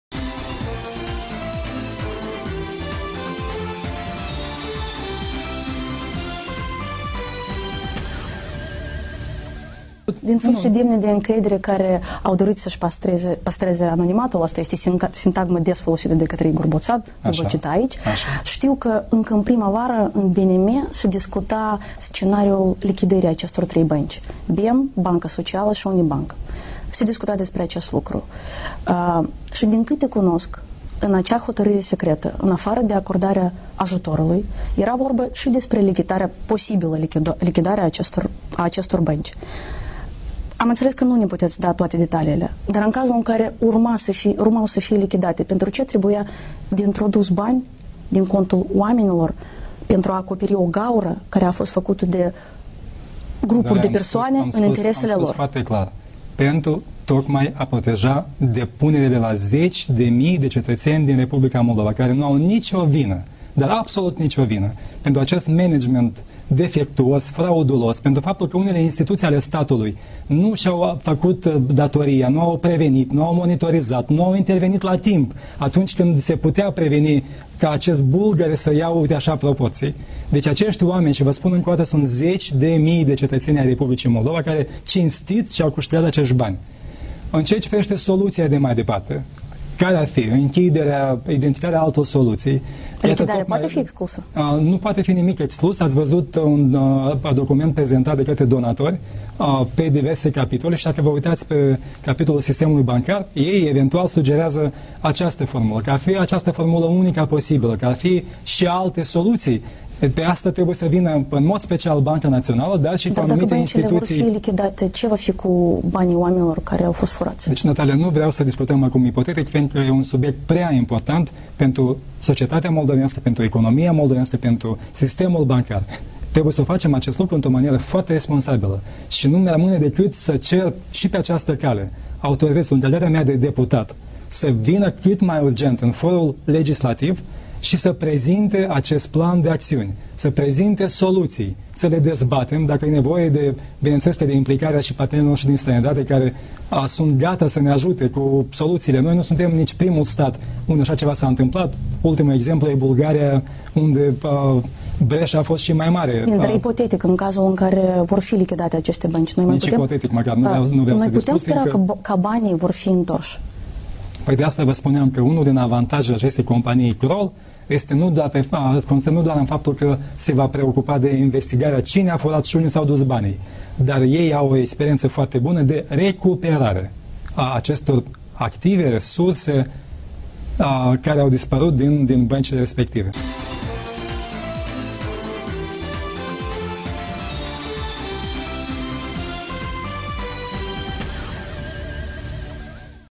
Что станет с деньгами вкладчиков, если Banca de economii будет ликвидирован
Молдавские политики допускают ликвидацию Banca de economii, однако отказываются говорить, что станет со вкладами граждан Молдовы, доверивших свои сбережения банку.